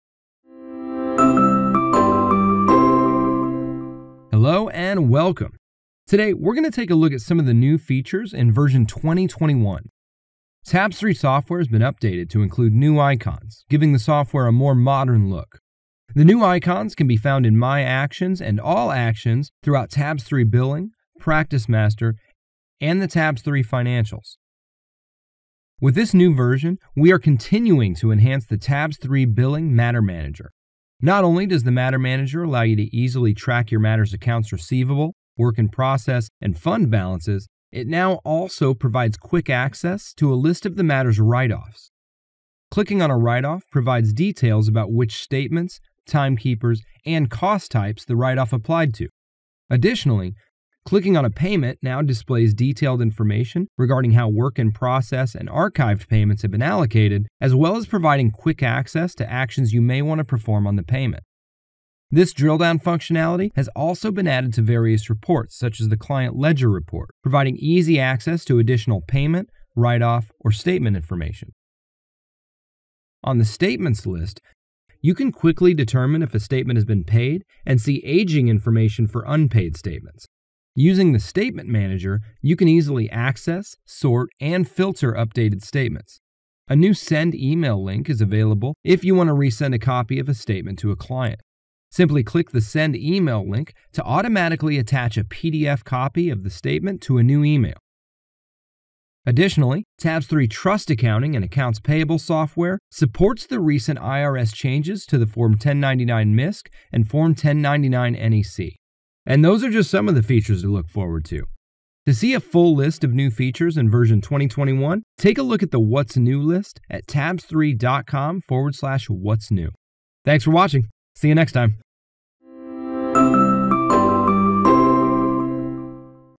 What's New in Version 2021
Check out some of the new features in Tabs3 Software Version 2021.